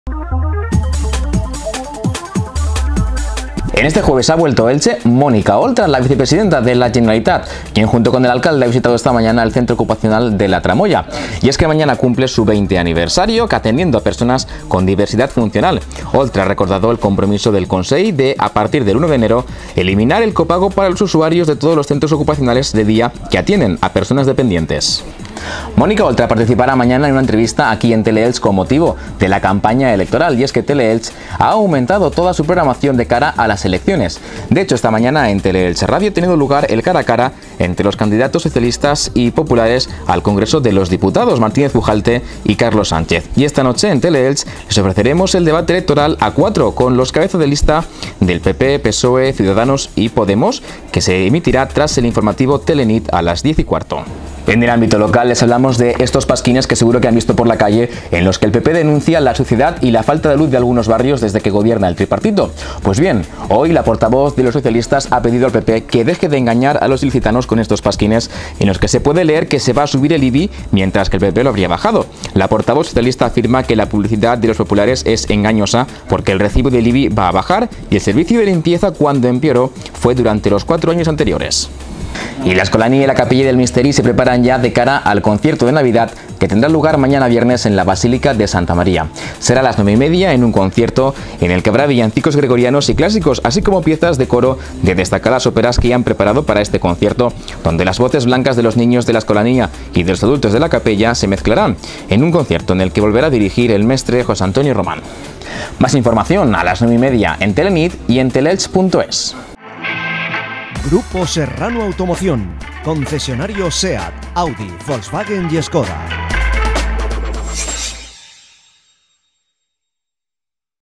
TN AVANCE | 17 DIC
Te resumimos en menos de dos minutos cómo va la campaña en Elche y otras noticias locales